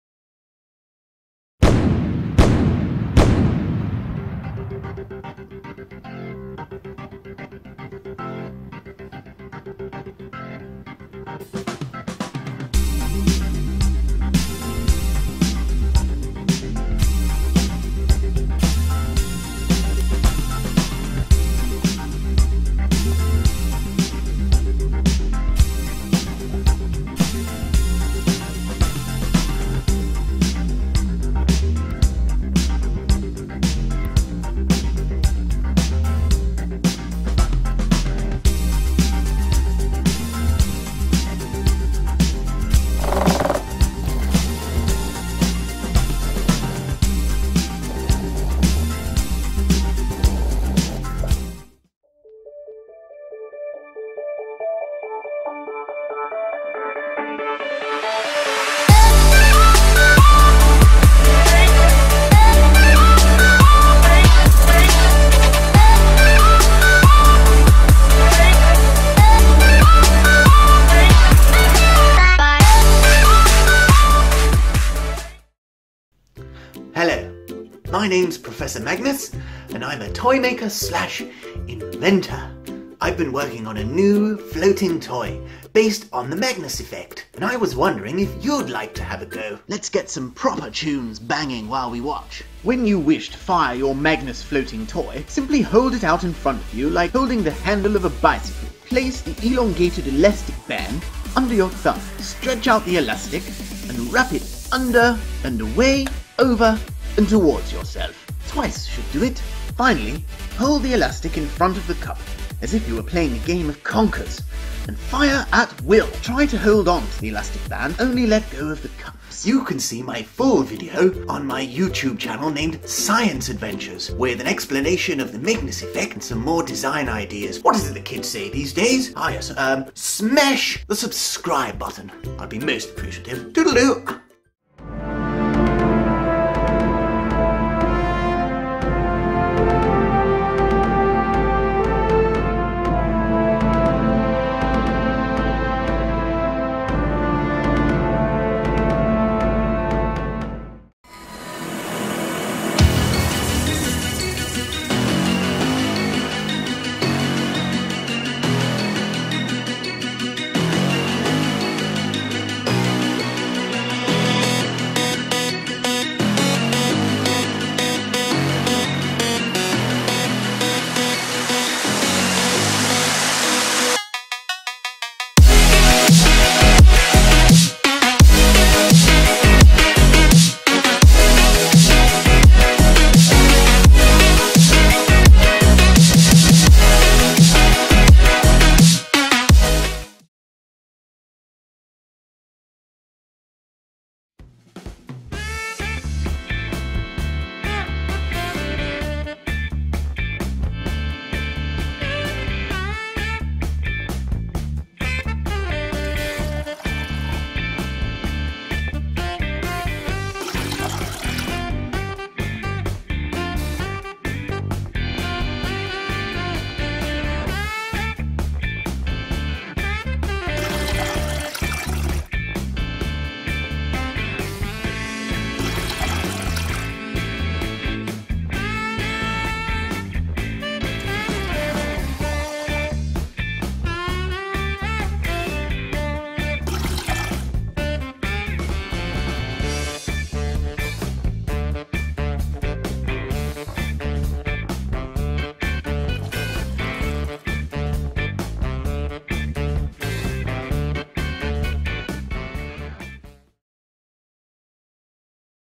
FIVE STEM CHALLENGES - In Under 5 Minutes
A compilation of some of our recent STEM based experiments for you to try with your students at school or your kids at home!

1. How to make a Scribble-Bot

2. Popsicle stick catapults

3. Magnus cups

4. Paper column challenge

5. Die hard challenge